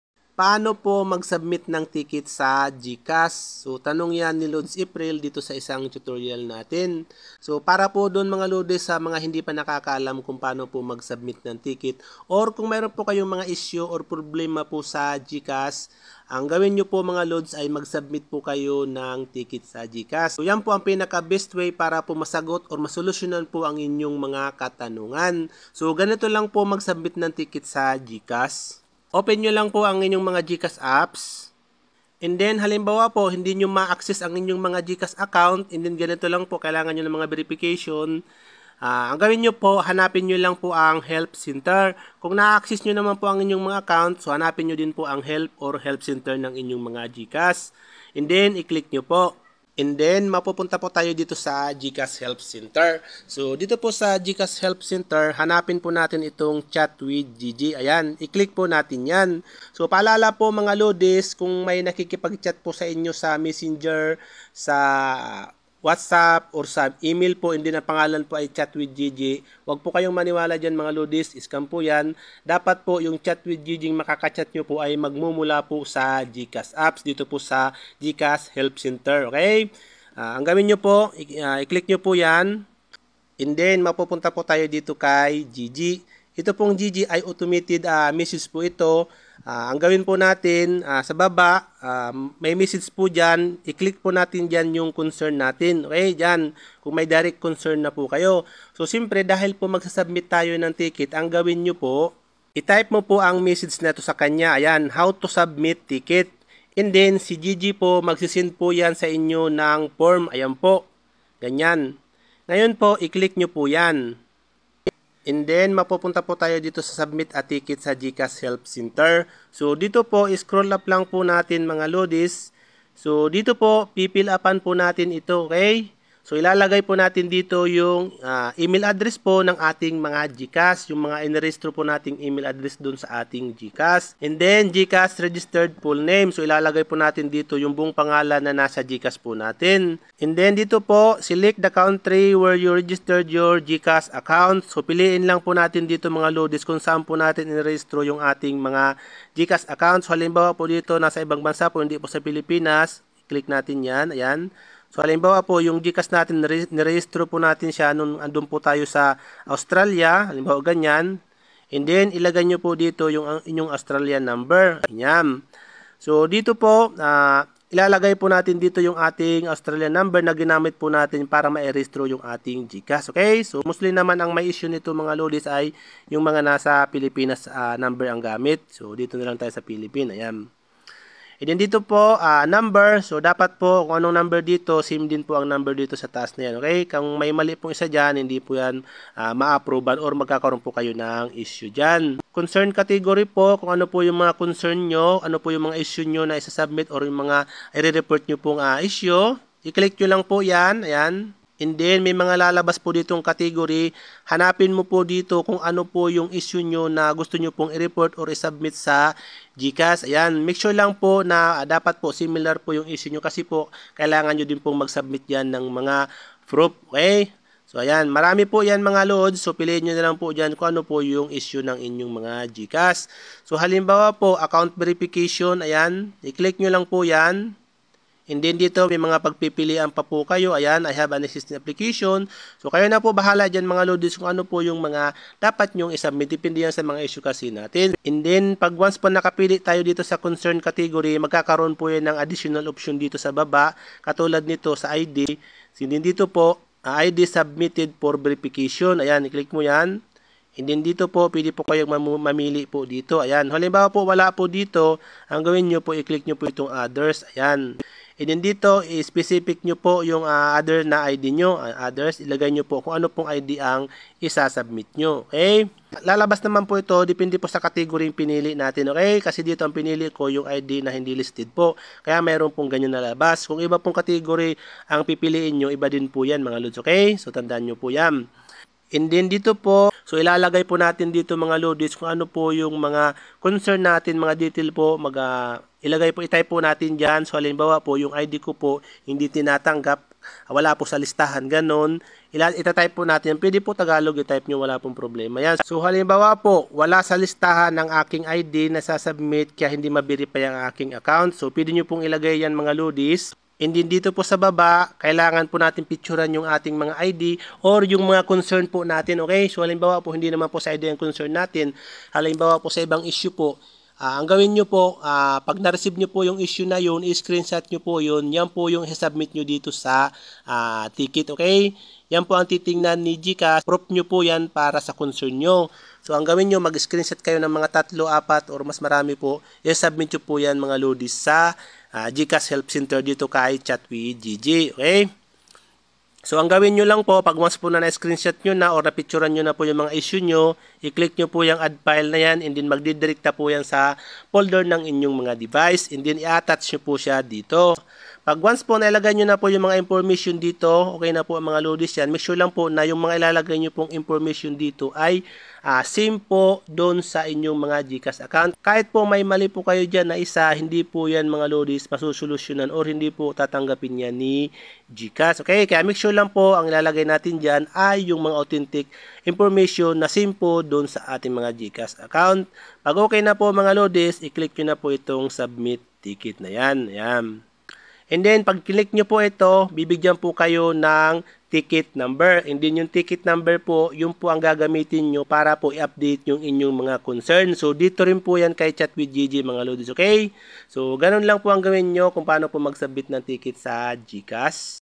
Paano mag Submit ng Ticket sa GCash?
Ganito ang tamang pag SUBMIT ng TICKET sa GCash para hindi ma SC@M. Madami ang na SC@M dahil sa maling pag SUBMIT NG TICKET sa GCash. Dito kayo mag Submit a Ticket, SURE at 100% LEGIT.